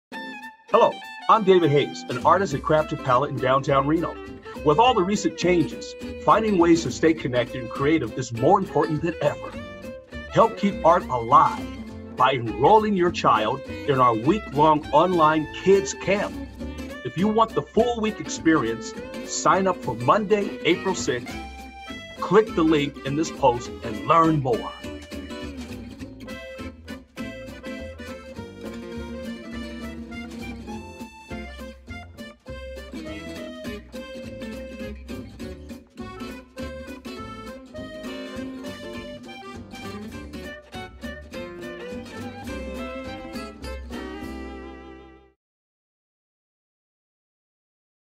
Keep Art aLIVE Commercial Spot with David Hayes Final
Kid’s going stir crazy?

Entertain their creativity by sending them to Keep Art aLIVE - Virtual Kid's Camp hosted by Crafted Palette!

Classes will be held online Monday, April 6th thru Friday, April 10th

        • 1:00pm-2:30pm Ages 6-10
            Theme: Music
            Taught by: David Hayes

        • 3:00pm-4:30pm Ages 11+
            Theme: Video Game Characters
            Taught by: Mina Hoskins

Classes are $15 per day or $60 for the week plus the cost of supplies.

A computer, laptop, or smart TV with internet access not included.

For an additional $5 per class or $20 per week, you will also receive an art kit containing everything your child needs to participate!

This includes canvas(es), easel, paintbrushes, paint, as well as an activity and a snack for each day!

We are doing everything in our power to continue to support our artist community during this uncertain time.

By participating in these courses, you are not only giving your child the gift of creativity, but you are also supporting artists whose livelihoods have been disrupted with our inability to host painting parties for the community.